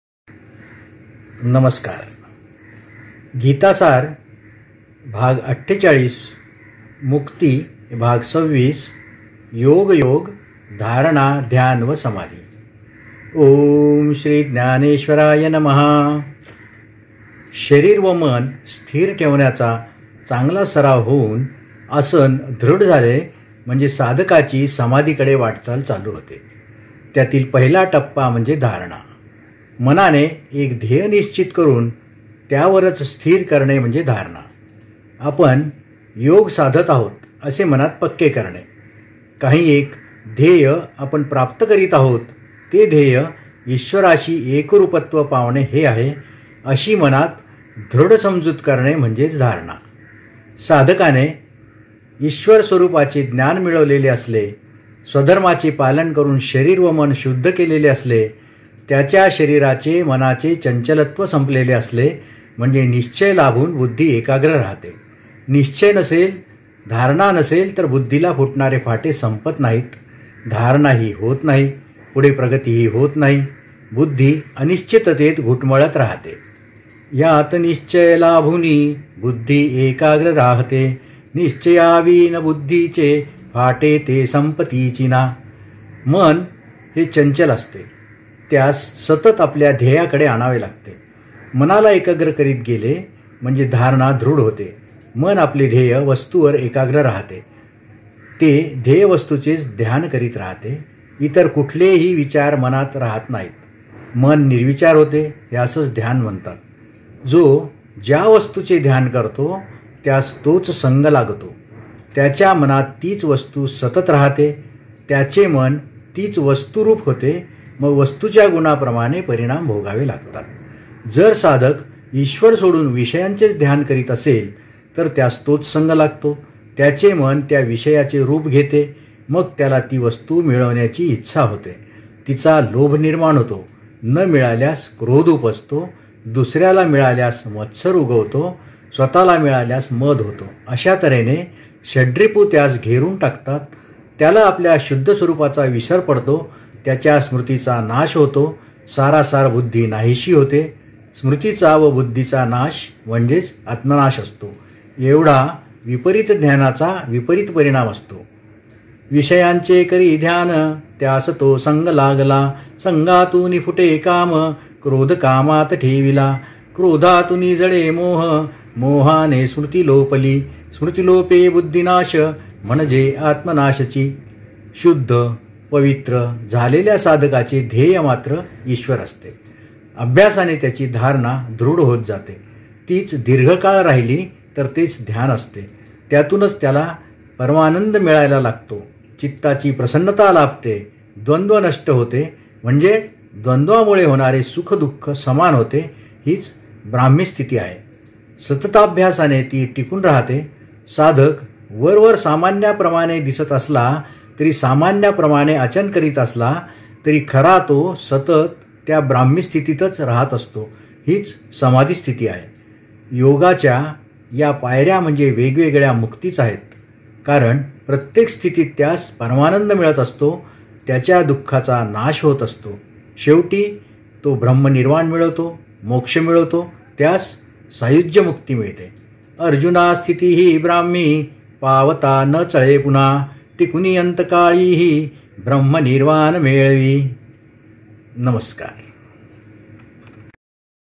Geeta Saar Part 48 Mukti Part 26
गीतासार भाग ४८ मुक्ति भाग २६.
या भागात आपण  गीतेमधील मुक्ती व मोक्ष मिळवण्याच्या मार्गांपैकी अष्टांग योगाच्या धारणा, ध्यान, समाधी या अंगाची ओळख  पाहणार आहोत.
यात आधारभूत असलेले श्लोक अनुक्रमे असे आहेत.
श्रीगीता २.४१,२.६२, २.६३, २.७२,